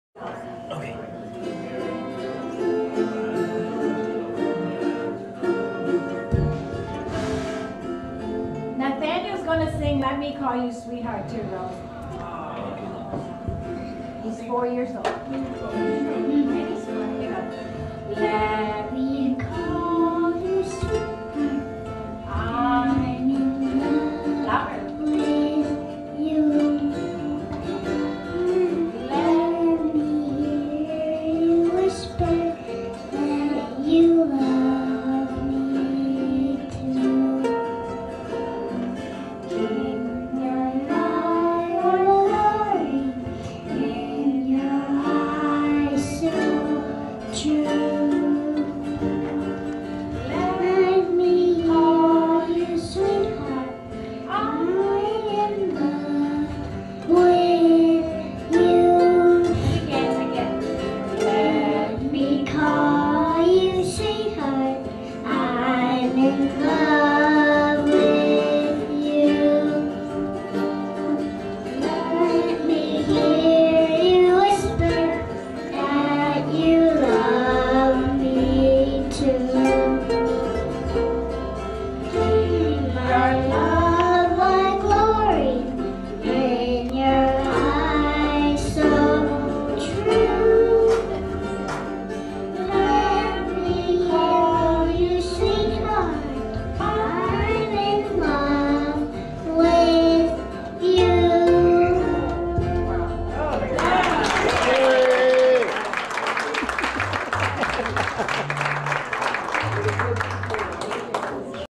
Hawaiian boy sings to Aunt Rosie
At Rosie's 101st birthday party, a young boy serinaded Aunt Rosie.